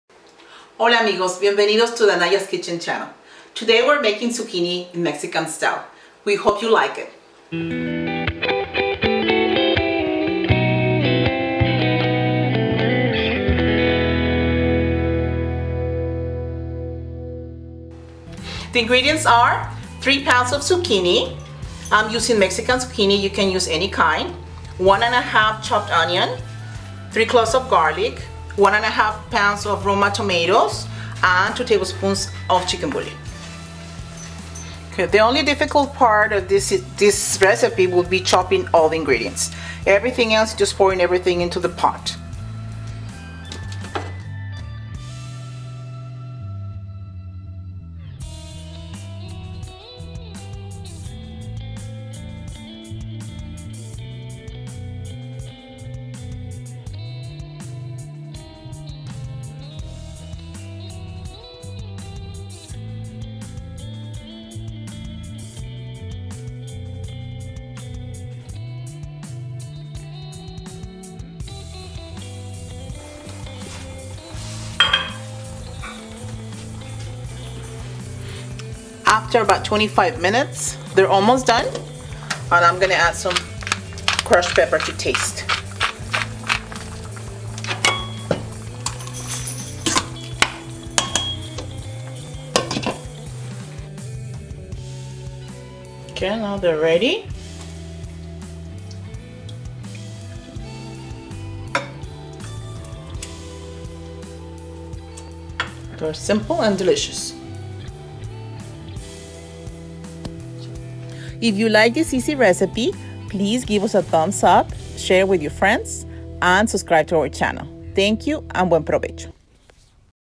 Mexican Style Zucchini
Five minute preparation, 20 minute cooking time super healthy and delicious side dish
Calabazas a la Mexicana